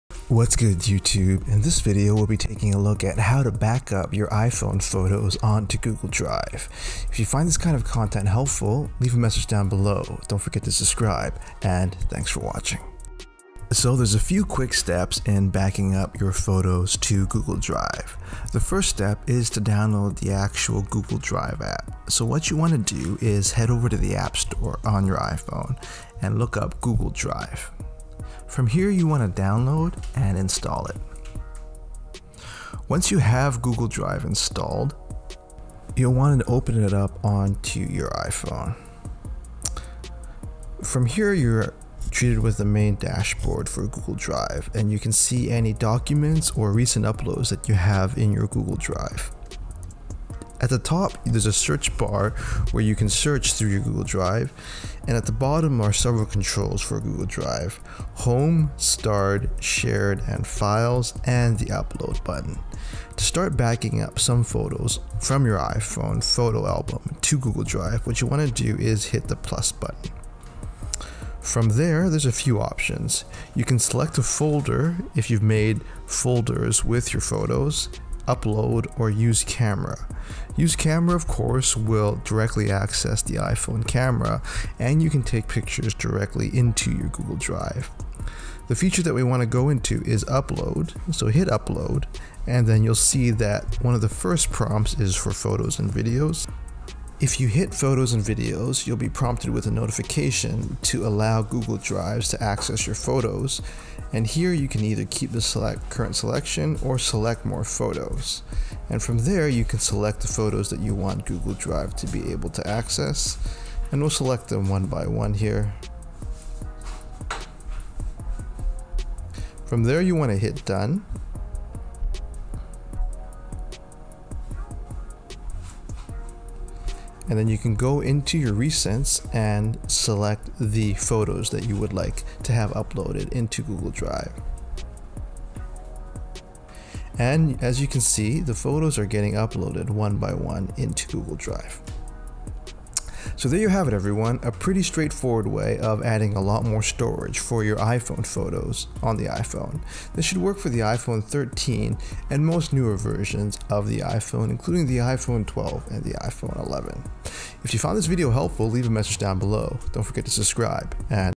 How to Backup iPhone photos to Google Drive | iPhone 13.
This video will go over how to backup your iPhone photos onto Google drive